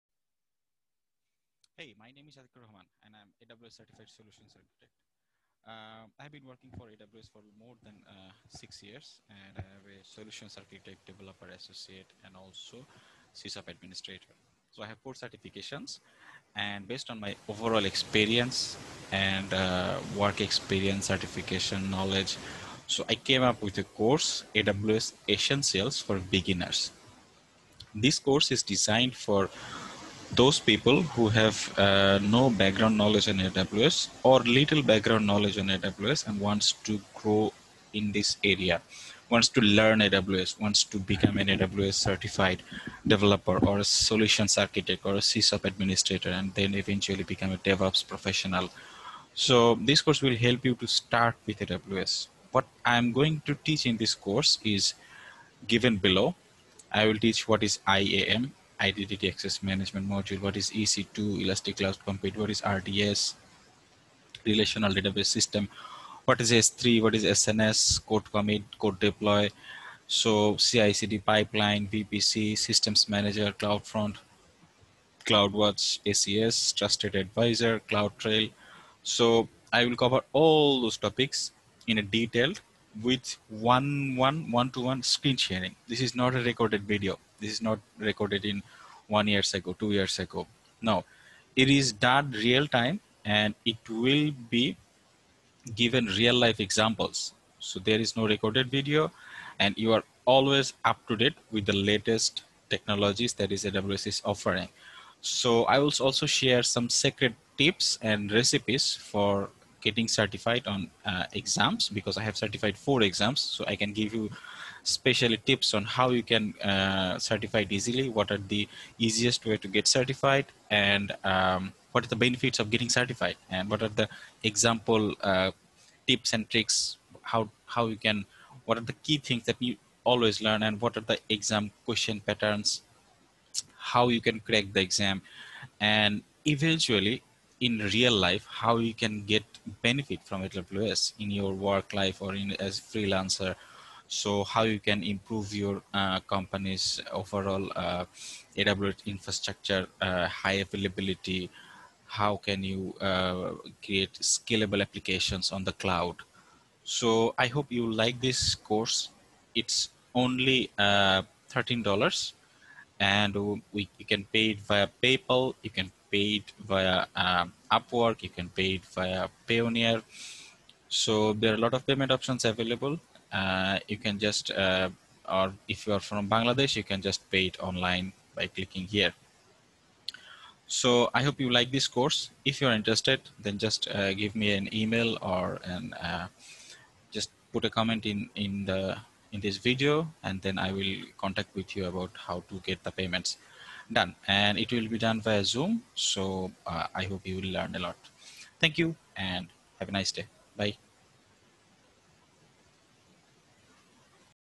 AWS Essentials for Beginners - Course Summary
This is a summary of my online course "AWS Essentials for Beginners".

The course fee is $12.99

You can pay me via Upwork as well.
This is my UpWork Profile URL

Course full details are given below